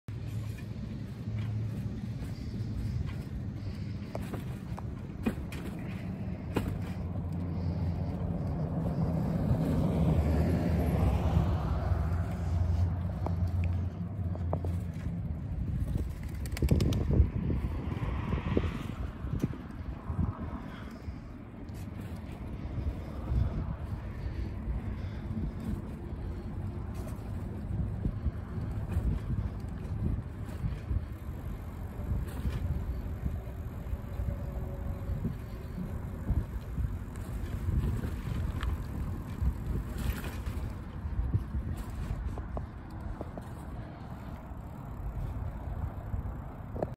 900 W to 800 W Folsom Trail
Sidewalk to Folsom Trail, heading toward Downtown.